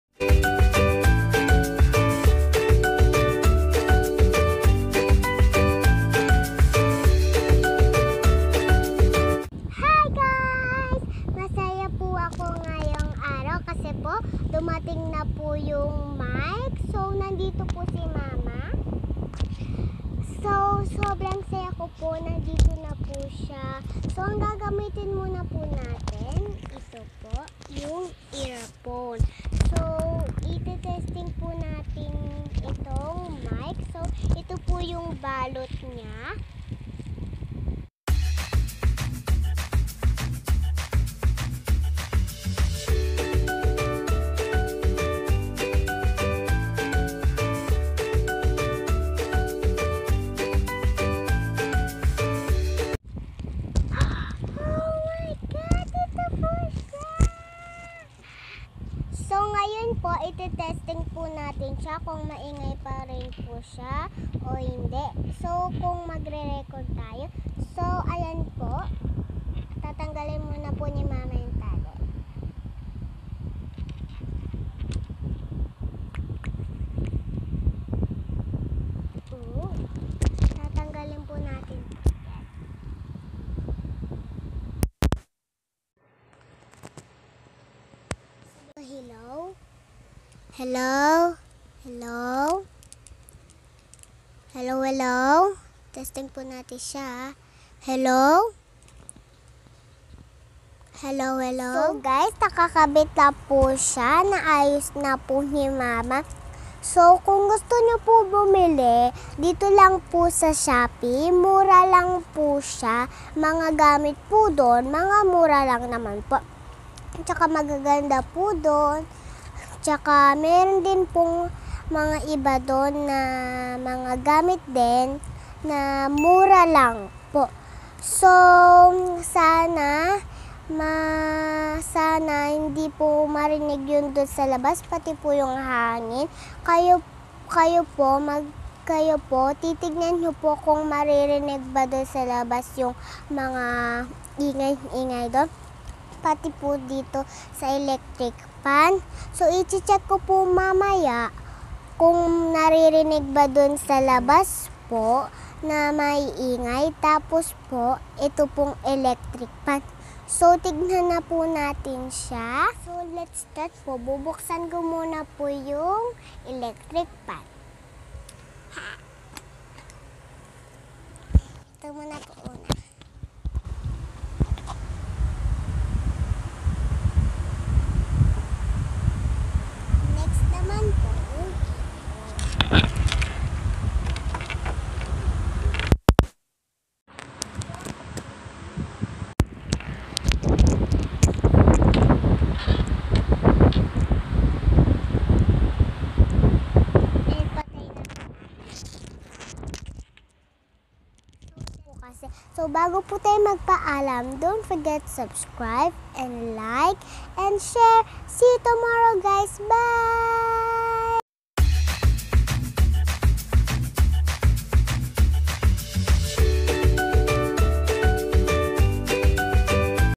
Mini-Clip mic (85 Pesos Only) is it worth it? 💗
Hi Guy's (Shin Aganan) here,. let's test my new Mini Clip mic if it's worth it or not? Come on Guy's let's find out! 🤩 watch my very satisfying Video,.thank you so mch guys 💗